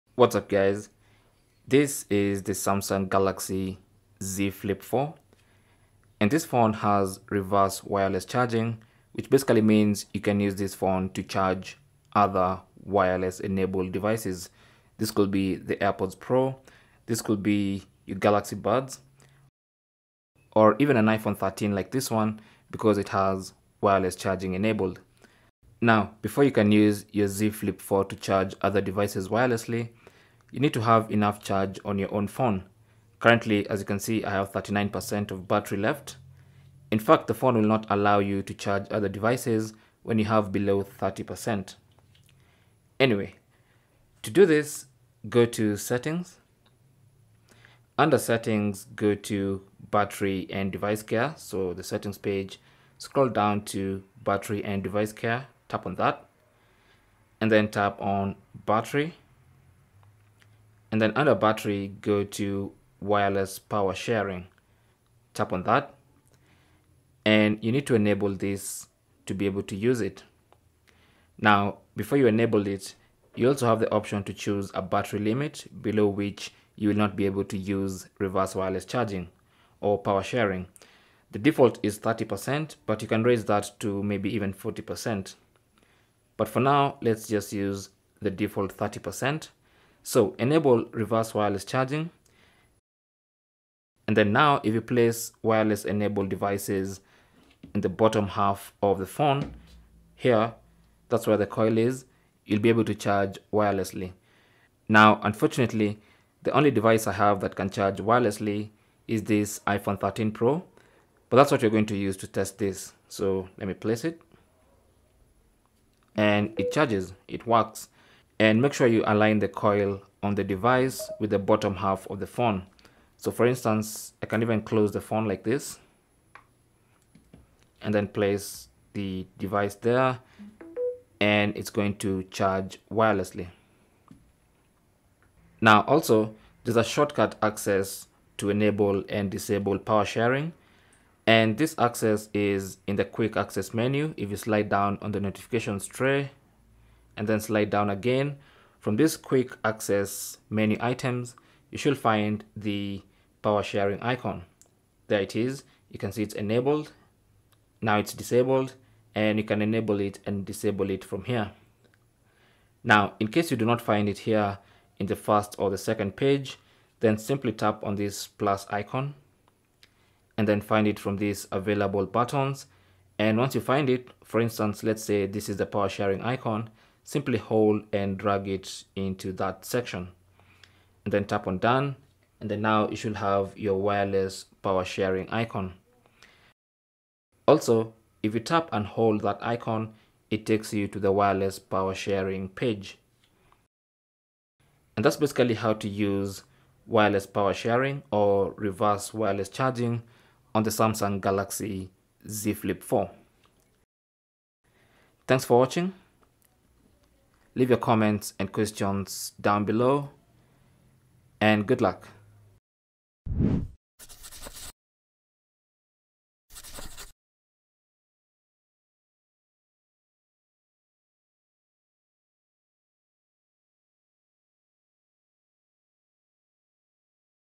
How To Enable Wireless Power Sharing On Samsung Galaxy Z Flip4
Learn how to enable and use Wireless power sharing on Samsung Galaxy Z Flip 4. Wireless power sharing is Reverse wireless charging.

Once enabled,  you can use your Galaxy Z Flip 4 to charge other Wireless enabled devices like phones,  earbuds,  Galaxy buds,  Airpods pro e.t.c.

You will also learn to adjust the battery limit below which your phone will not allow Wireless power sharing limit.

SAMSUNG Galaxy Z Flip 4 (Factory Unlocked)

Cell Phone Tripod Adapter